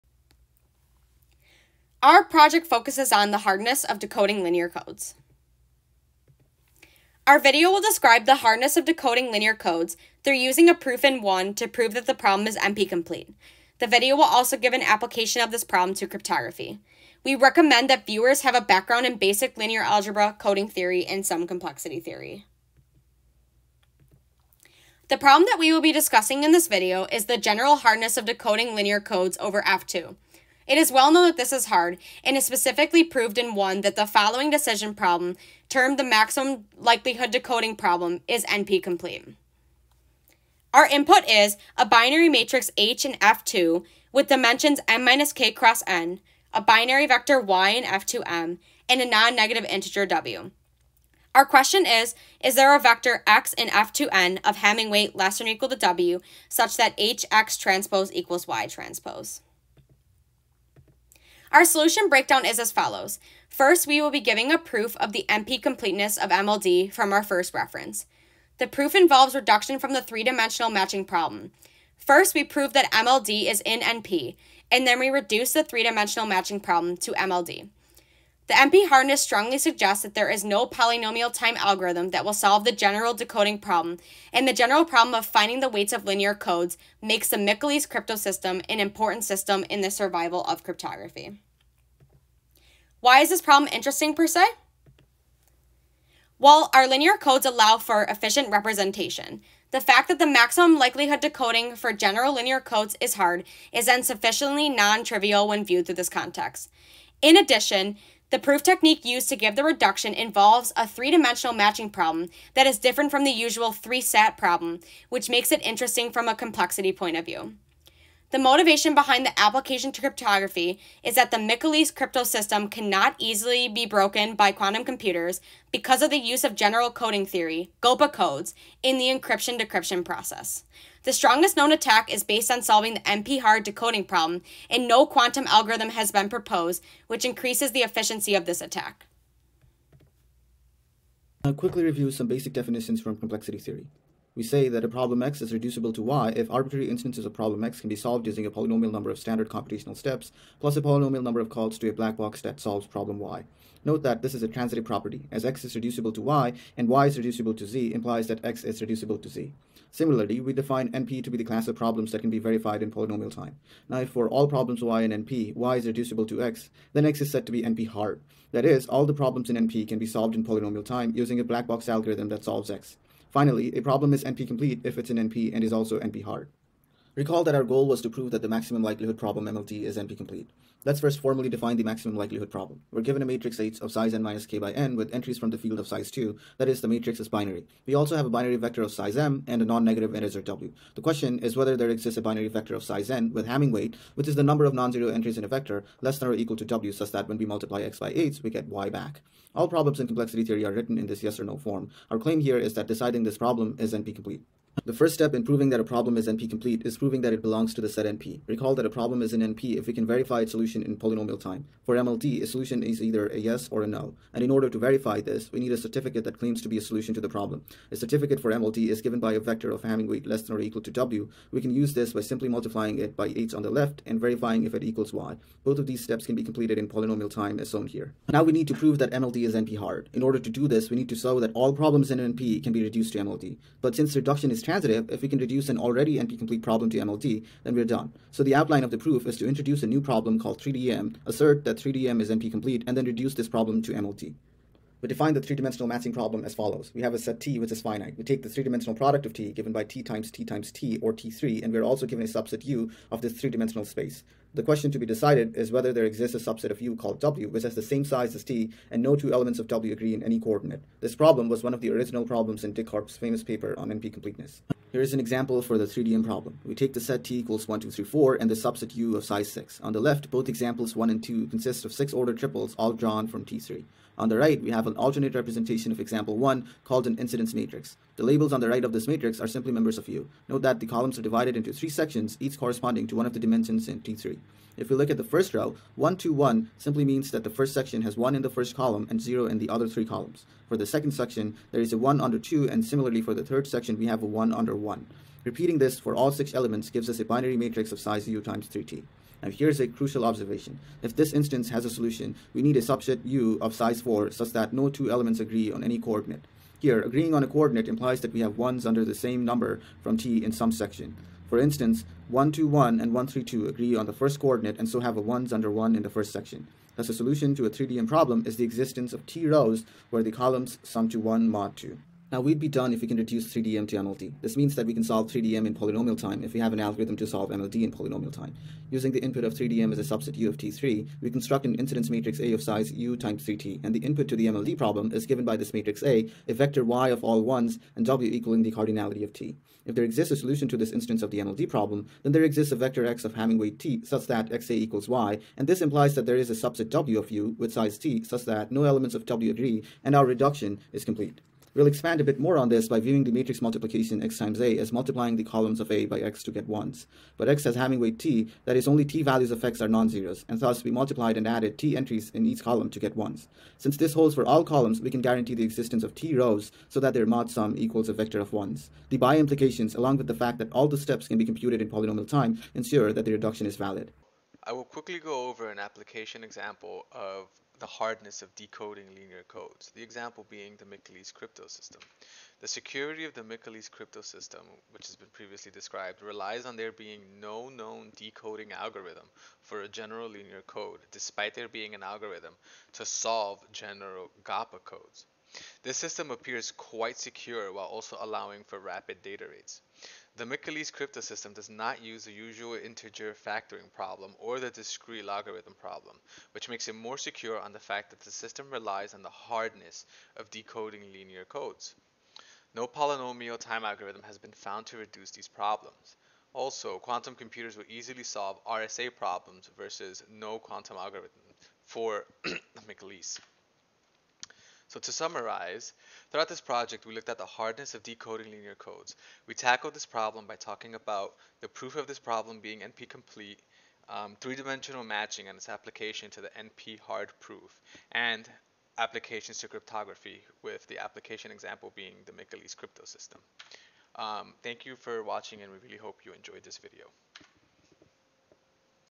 Decoding (Linear) Codes is Hard!
We go over the proof of NP-Completeness of the Maximum Likelihood Decoding Problem from Coding Theory, and discuss its potential application in Quantum Cryptography.